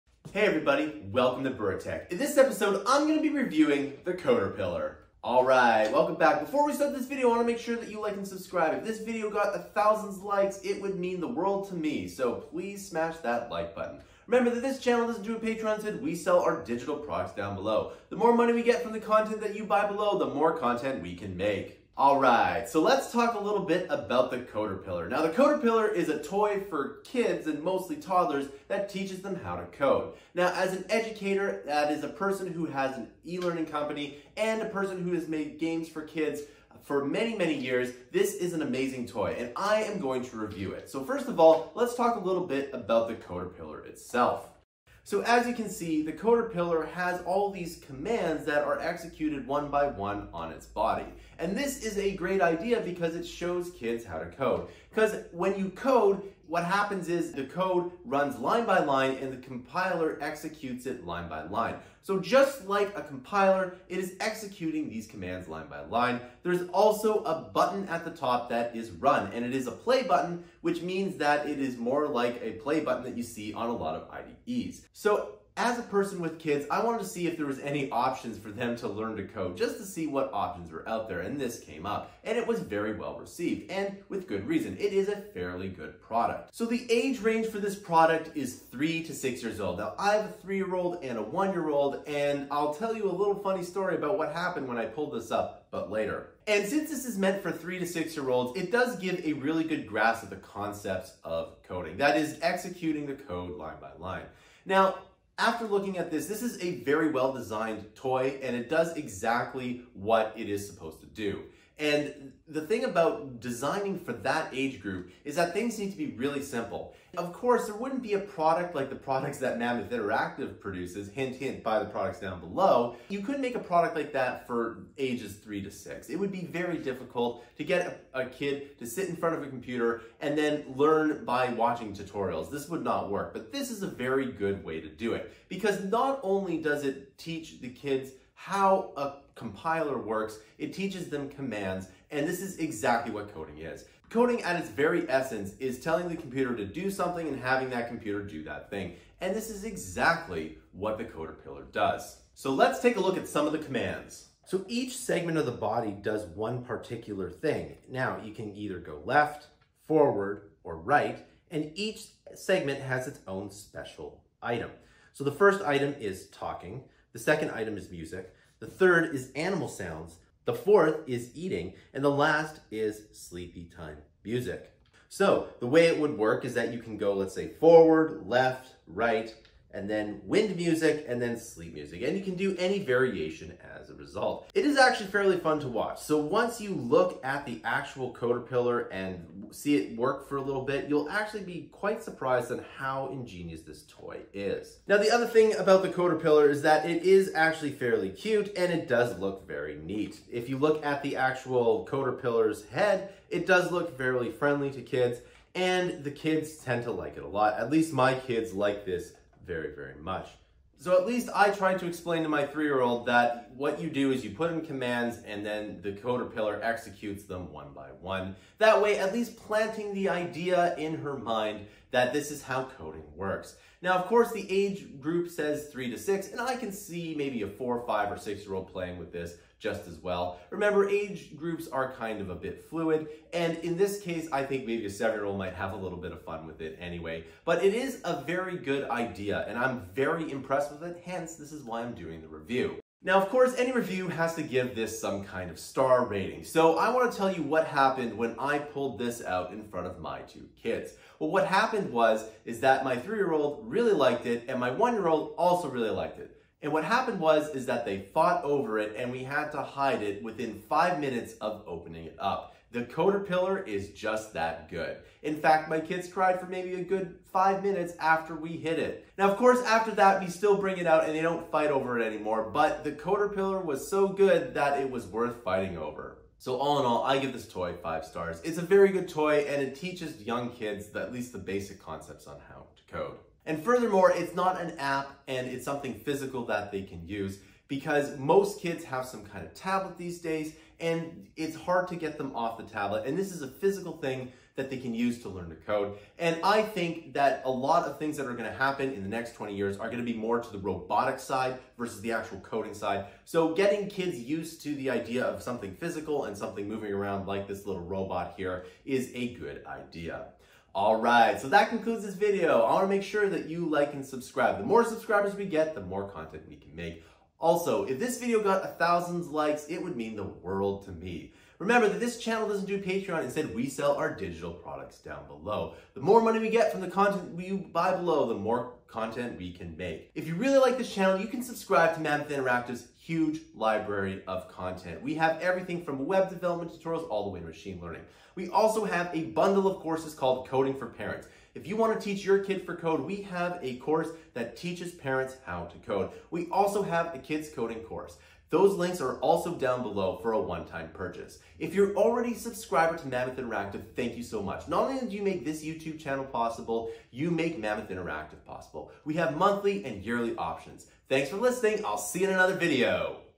Code-a-Pillar Review - Coding for Kids
Today we review the Code a Pillar Twist by Fisher Price. Teach your kid how to code with this toy for toddlers...

~John Bura